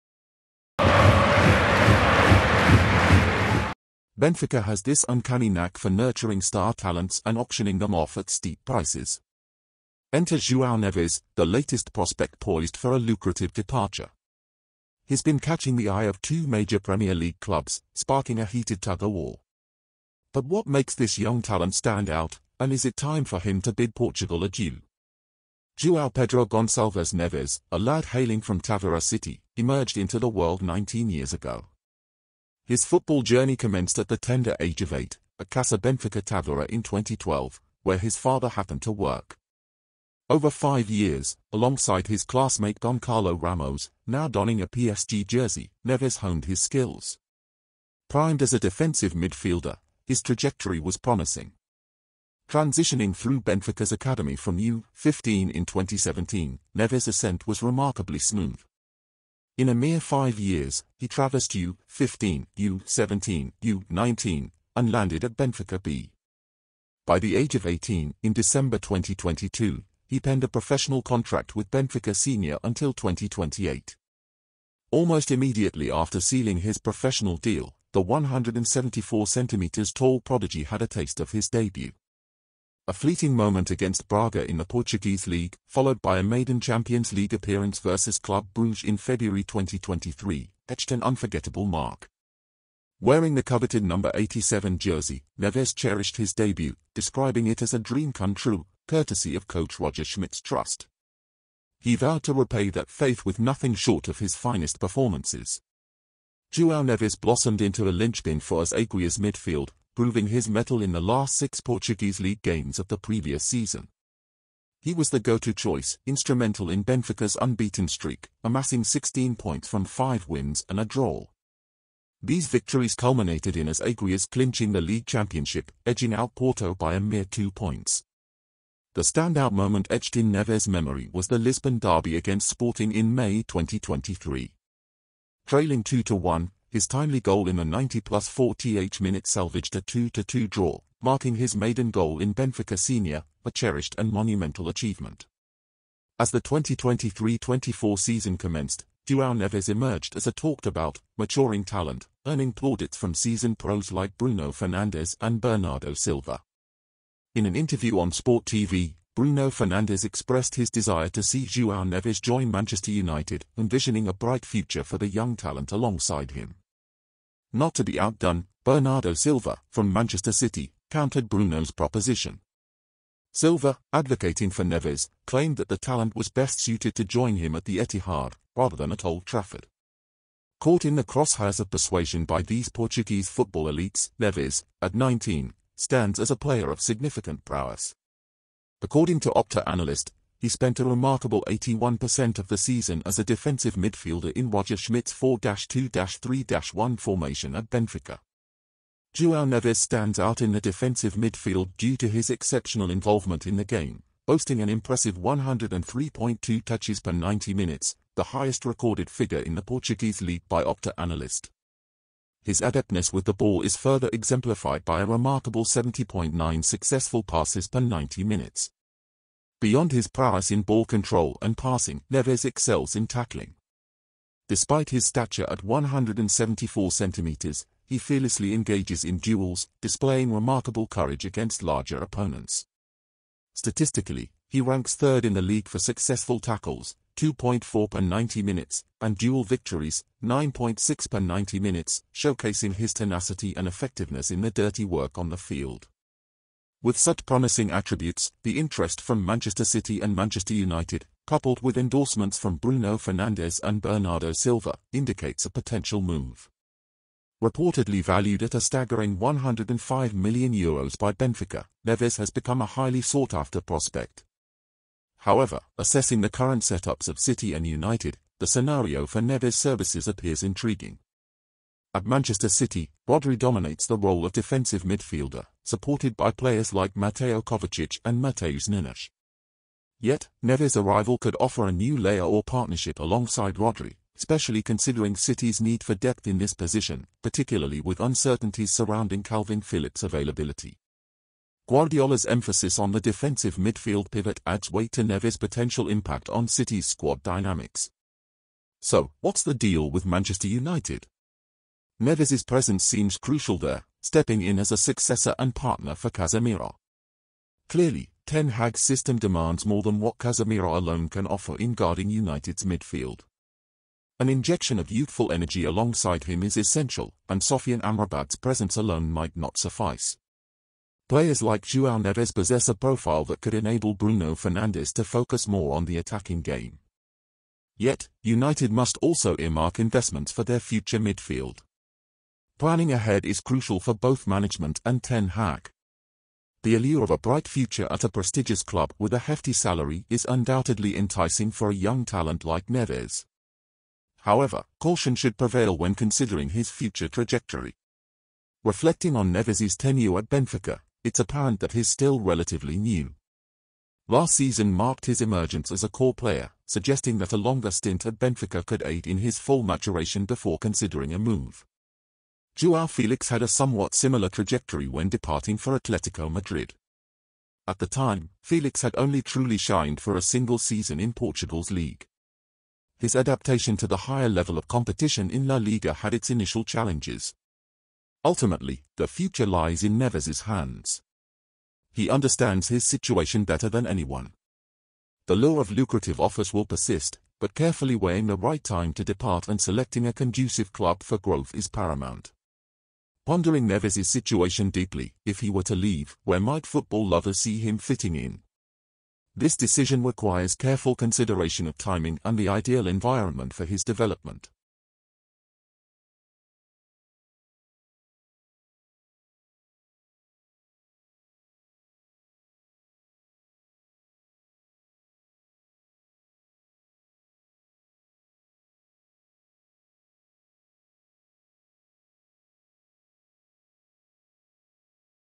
The Battle for Manchester: Assessing Joao Neves's Impact and Potential!
Benfica has this uncanny knack for nurturing star talents and auctioning them off at steep prices. Enter Joao Neves, the latest prospect poised for a lucrative departure. He's been catching the eye of two major Premier League clubs, sparking a heated tug-of-war. But what makes this young talent stand out, and is it time for him to bid Portugal adieu?

Joao Pedro Goncalves Neves, a lad hailing from Tavira City, emerged into the world 19 years ago. His football journey commenced at the tender age of 8, at Casa Benfica Tavira in 2012, where his father happened to work. Over five years, alongside his classmate Goncalo Ramos (now donning a PSG jersey), Neves honed his skills.

Primed as a defensive midfielder, his trajectory was promising. Transitioning through Benfica's academy from U-15 in 2017, Neves' ascent was remarkably smooth.

In a mere five years, he traversed U-15, U-17, U-19, and landed at Benfica B. By the age of 18, in December 2022, he penned a professional contract with Benfica senior until 2028.

Almost immediately after sealing his professional deal, the 174cm tall prodigy had a taste of his debut. A fleeting moment against Braga in the Portuguese League, followed by a maiden Champions League appearance versus Club Brugge in February 2023, etched an unforgettable mark.

Wearing the coveted number 87 jersey, Neves cherished his debut, describing it as a dream come true, courtesy of coach Roger Schmidt's trust. He vowed to repay that faith with nothing short of his finest performances.

Joao Neves blossomed into a linchpin for As Aguias' midfield, proving his mettle in the last six Portuguese League games of the previous season. He was the go-to choice, instrumental in Benfica's unbeaten streak, amassing 16 points from five wins...